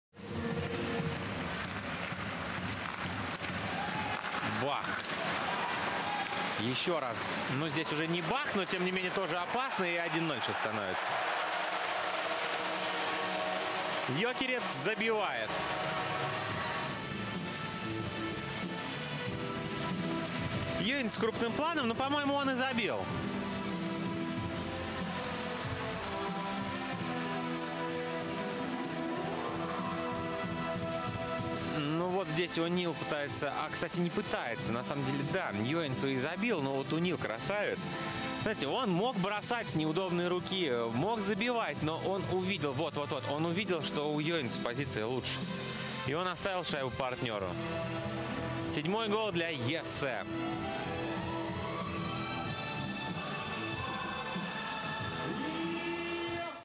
Йоэнсуу добивает в пустые ворота
Ессе Йоэнсуу открывает счет в ситуации, где ему откровенно повезло с отскоком шайбы